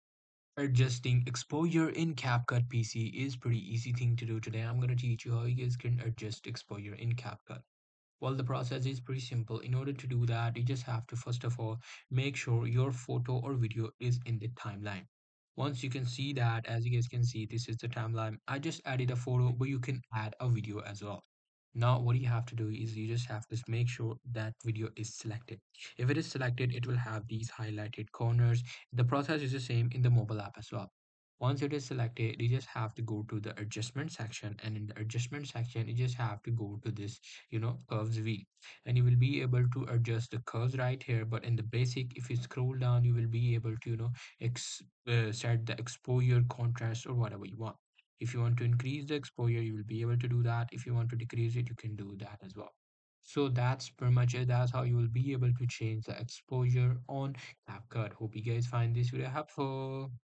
How To Adjust Exposure In Capcut (2024) - FULL GUIDE!
In this video I will show you how to adjust exposure in capcut